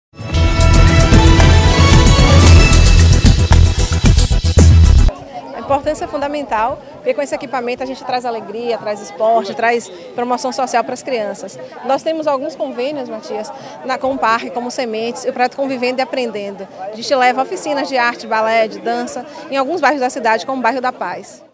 A importância do Parque da Cidade segundo titular da SEMPS Salvador, Ana Paula Matos
Notícias, Política, Salvador, Bahia, Brasil